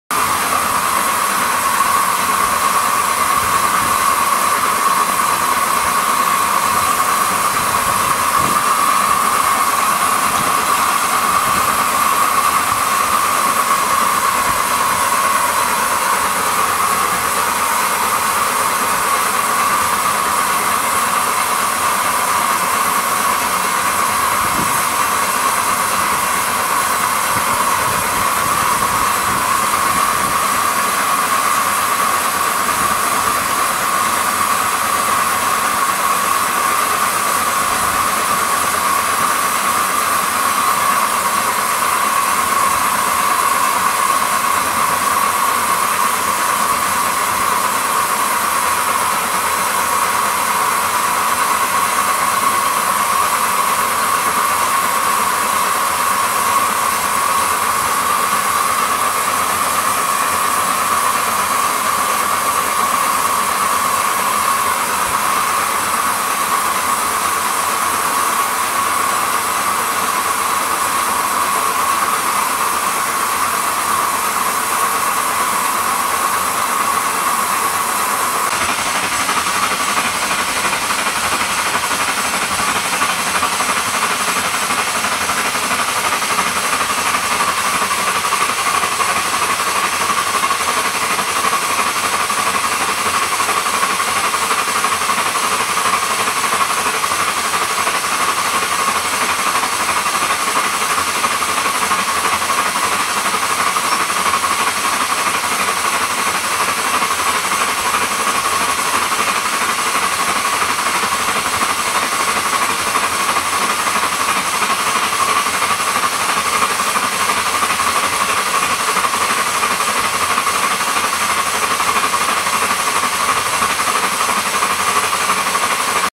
Live Steam NYC Dreyfuss Hudson
Bench testing 7,1/4 Live Steam New York Central Dreyfuss Hudson